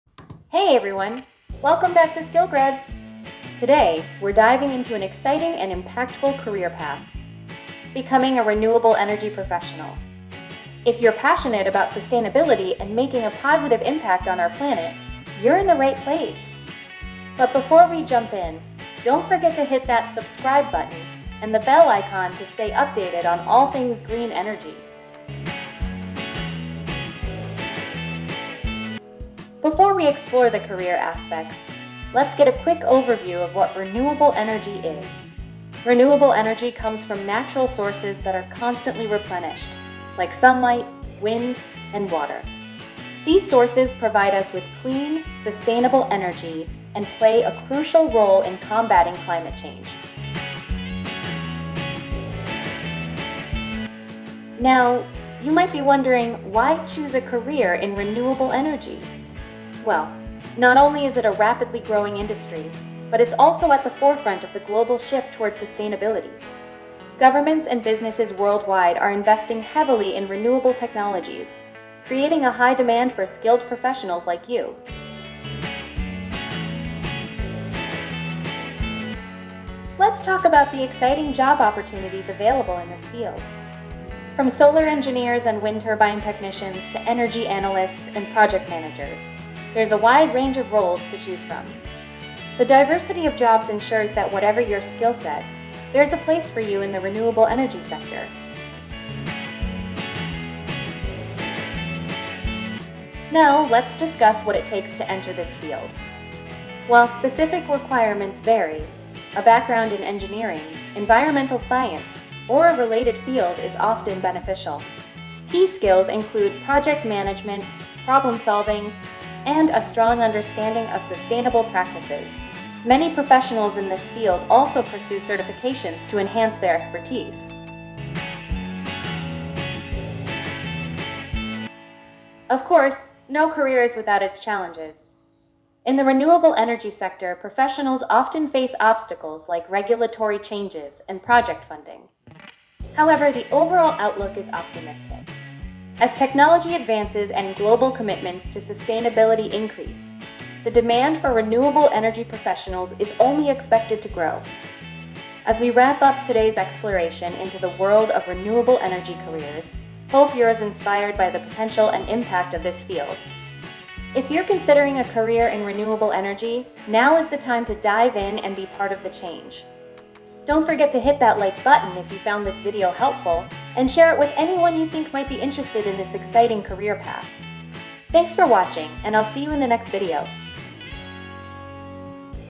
Career Options : A Career as a Renewable Energy Professional | Unlock the future
Explore the dynamic world of Renewable Energy with our latest video! Join us as we delve into the exciting opportunities and impactful careers in the sustainable energy sector. From wind farms to solar panels, discover how you can contribute to a greener future. Don't miss out on valuable insights from real-life success stories and gain a glimpse into the challenges and bright future of renewable energy. Ready to be a part of the change?